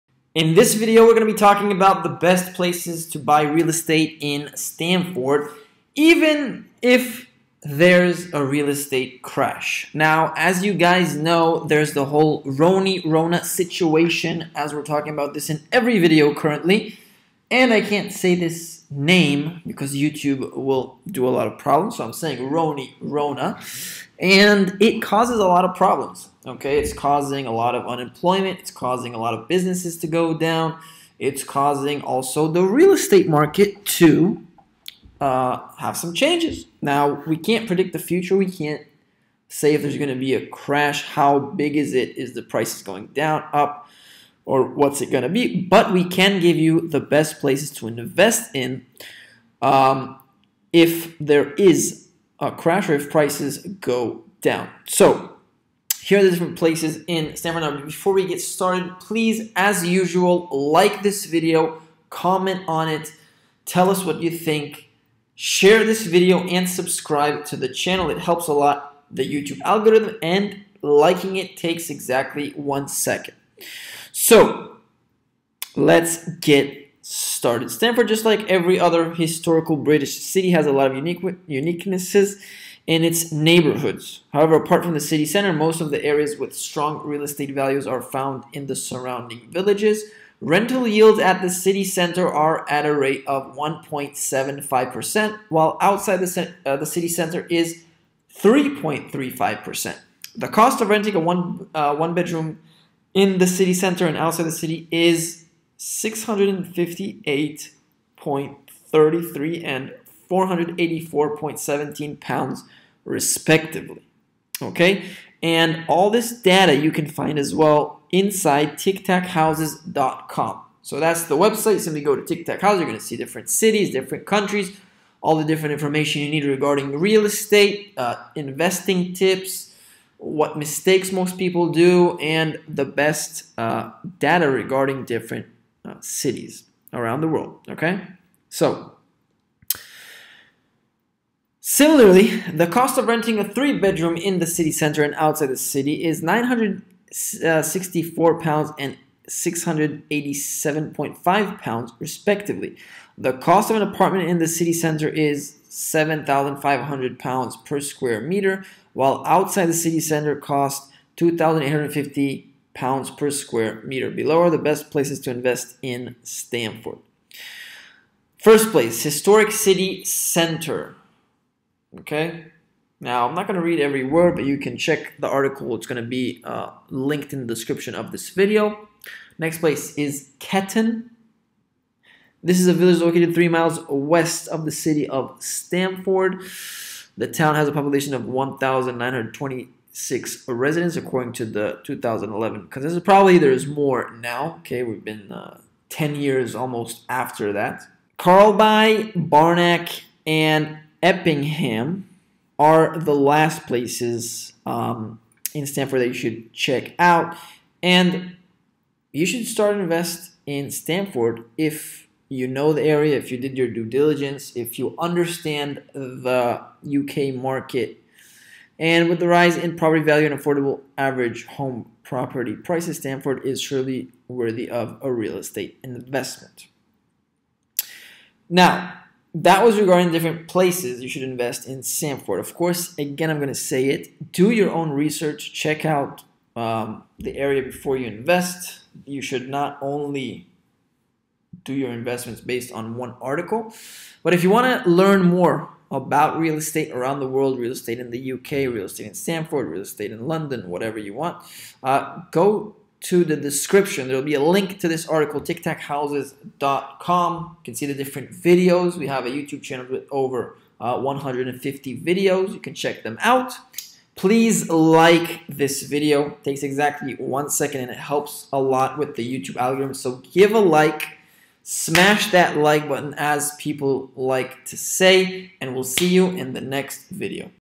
Real Estate Updates In Stamford, Historic City Center, Ketton, Ryhall, Carlby, Barnack & More.
If you are looking forward to investing in any location, you need to have a better knowledge of the neighborhoods and nearby locations. That is one of the deciding factors for property investment.

Stamford, just like every other historical British city, has a lot of uniqueness in its neighborhoods. However, apart from the city center, most of the areas with strong real estate value are found in the surrounding villages. Rental yields at the city center are at a rate of 1.75% while outside the city center is 3.35%. The cost of renting a one-bedroom in the city center and outside the city is €658.33 and €484.17, respectively.

Ryhall
This is a small town located at the eastern boundary of the Rutland County. It is situated approximately two miles north of the city of Stamford. As per the 2011 census, the population of the area is 1,614 residents. The village features a waterfront from the River Gwash. The transport system in the area is serviced by the

Carlby
This is a small town sitting on the Stamford to Bourne A6121 main road. The area has a population of 542, according to the 2011 census. There are playing fields, green parks with trails, and other social amenities, including a library, small shops, and a post office.

Barnack
We have arrived at one of the most picturesque and beautiful parts of Stamford. It is located 3.5 miles southeast of the city of Stamford. Perfectly situated in between two cathedrals, this suburb offers the best cosmopolitan lifestyle coupled with a taste of architectural pleasure. This suburb got its name as a result of the attractive Georgian townhouses that decorate the cobbled street.

Empingham
An area saturated by local amenities, nurseries, primary and secondary schools, Empingham is a place where families thrive. The population of the area is 872, according to the 2011 census. This small suburb boasts of about twelve schools, and many young couples migrate to this neighborhood, searching for metropolitan bliss. They also consider the good schools in the area to be advantageous.